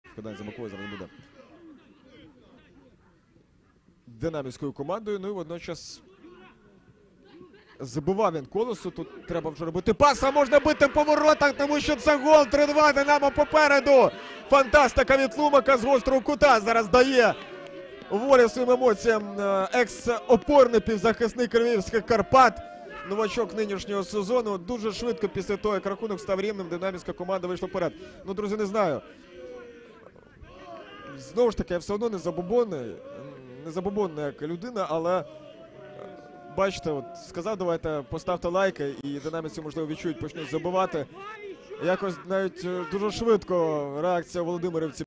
СУПЕРГОЛ! ТЛУМАК! ІНГУЛЕЦЬ U19 - ДИНАМО U19 2:3
0:1 Волошин (15 хв) 1:1 Сейтхалілов (28 хв) 2:1 Лісняк (55 хв) 2:2 Волошин (74 хв) 2:3 Тлумак (75 хв)
ДИНАМО: 1 Ігнатенко, 22  Малиш, 25 Західний, 14 Дячук, 21 Танчак, 17 Тлумак, 6 Яцик, 18 Царенко, 19 Волошин, 10 Блізніченко, 23 Попов
Запас: 12 Ісаченко, 15 Опанасенко, 13 Філатов, 16 Мащенко, 9 Бенедюк, 24 Михайлів, 8 Жозе, 11 Горбач, 5 Скрипник
У суботу, 21-го листопада, відбудеться матч 10-го туру Чемпіонату України U19, у якому ІНГУЛЕЦЬ прийматиме київське ДИНАМО.
Поєдинок пройде у с. Володимирівка на полі стадіону ім. Олександра Поворознюка, початок о 13:00.